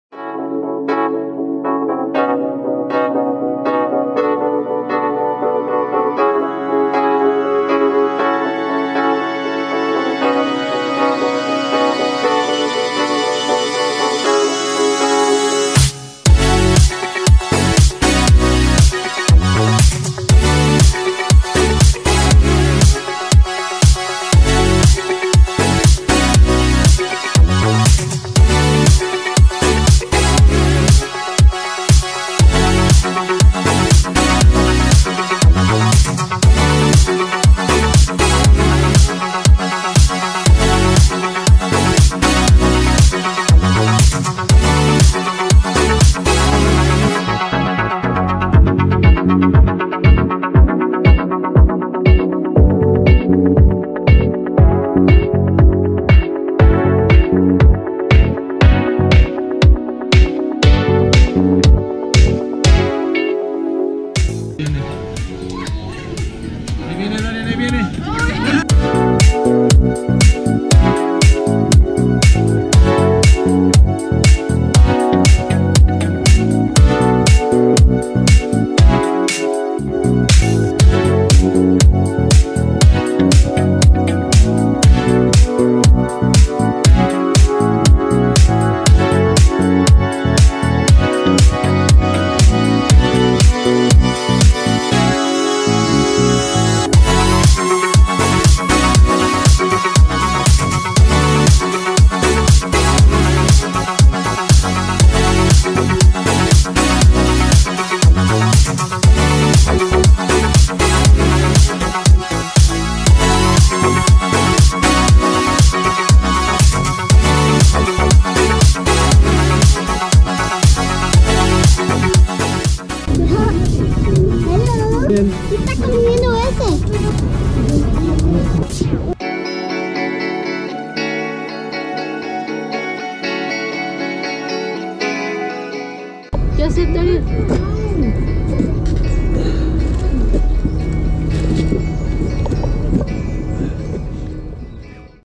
California Kids Visiting Woodland zoo in Seattle
Seattle Adventure Day 5 Cakifornia kids visit  Woodland Zoo in Seattle.

We could spy a Bear, Wolves, Eagles, and many more amazing animals!